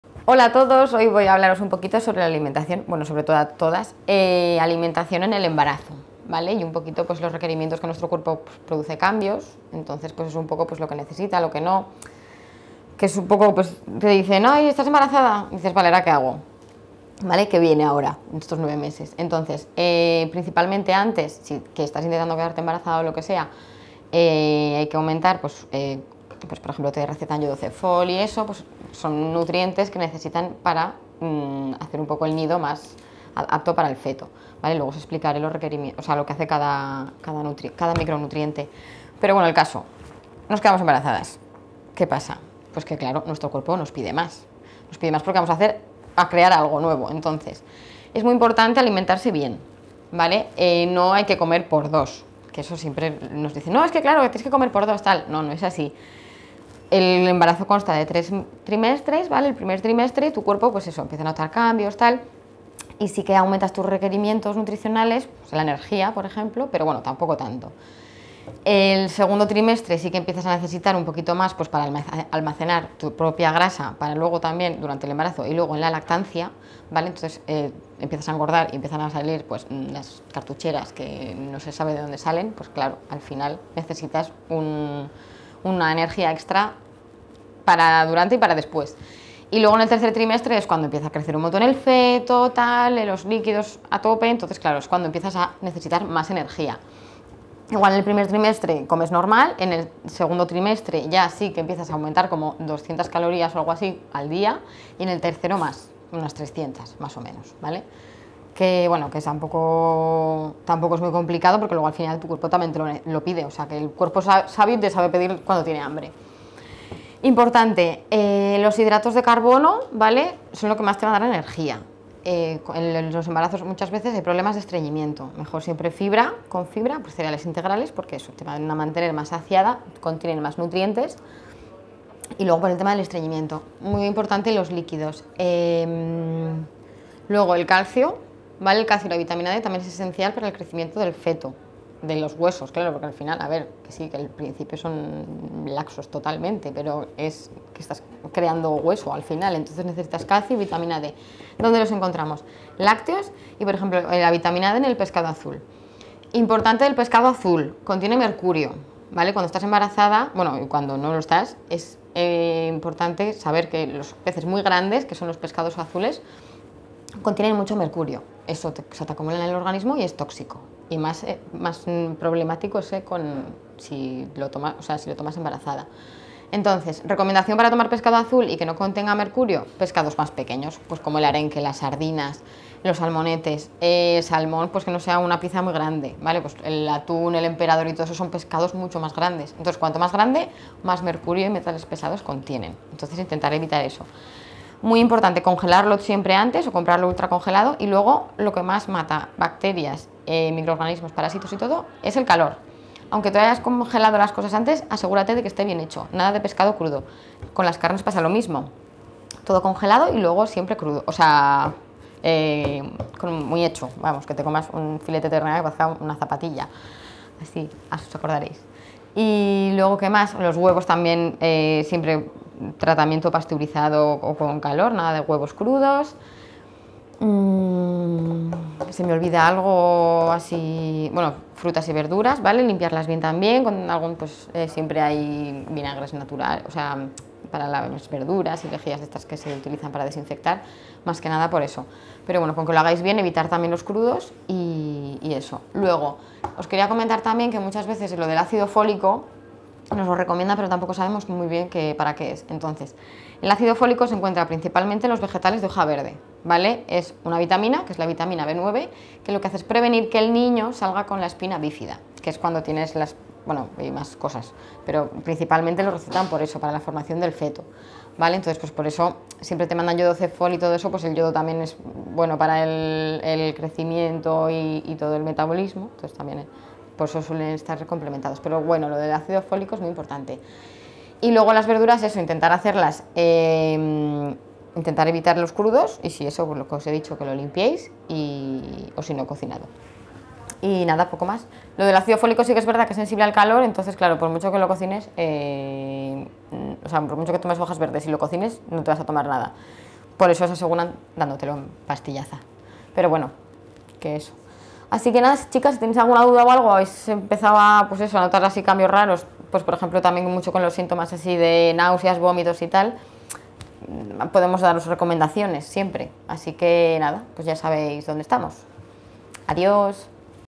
Trucos para afrontar la nutrición durante el embarazo
¡MAE MÍA LO DE CREAR VIDA! ¿Sabemos cómo afrontar la nutrición durante el embarazo? Hoy hablamos de los requerimientos durante el embarazo, y la diferencia en las fases que hay.